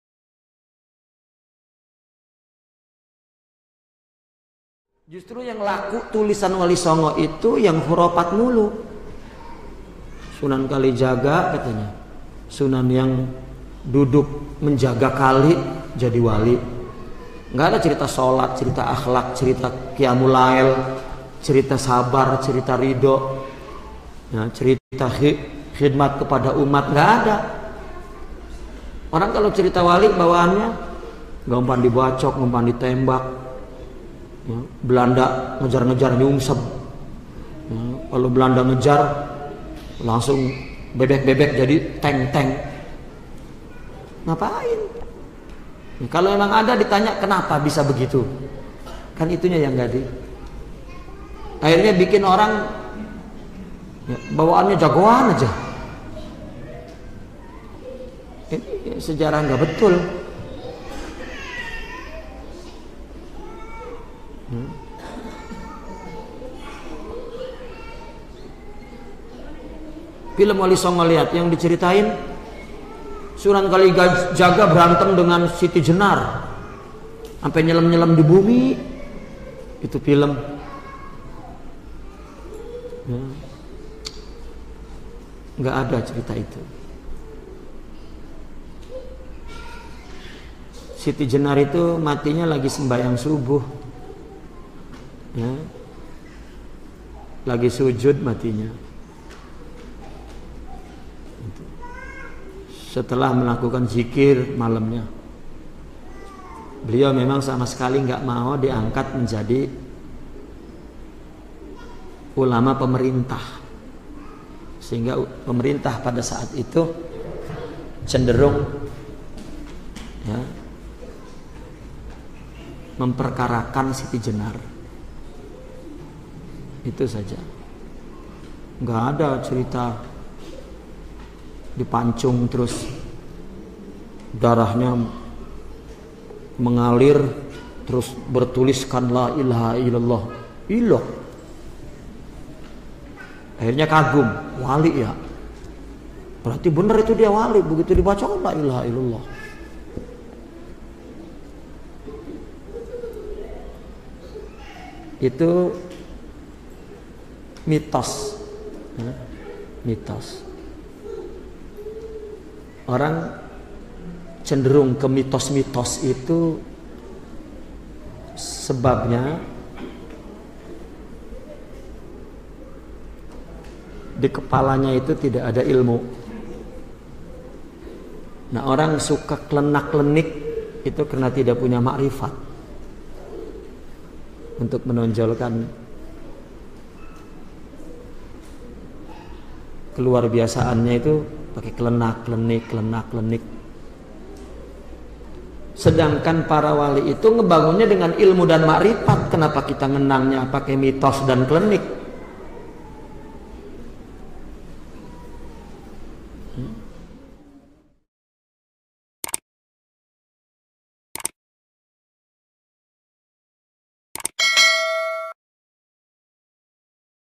Meluruskan Sejarah yang menyimpang dari kisah Wali Songo
Sedikit dikitnya Meluruskan sejarah yang menyimpang dari kisahnya wali songo, versi lengkapnya di video pengajian kitab Bariqul Anwar.